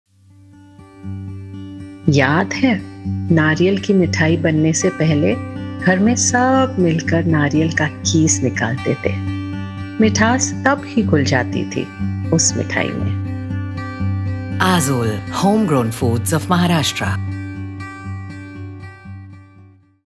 Nutritious Soft Chikki Made with Only 2 Ingredients!
Aazol's Soft Coconut Jaggery Chikki is a high-energy chikki handmade with just two natural ingredients - coconut and liquid jaggery.

What is so special about these 2 ingredients?

- Coconut is a nutrient-rich food that supports digestion, boosts immunity and improves heart health.
- Liquid jaggery is a natural sweetener that is rich in minerals and potassium, making it a healthier alternative to refined sugar.

Aazol’s Chikki is also free from artificial colours, flavours and preservatives, making it an excellent snacking option for people of all ages 😊.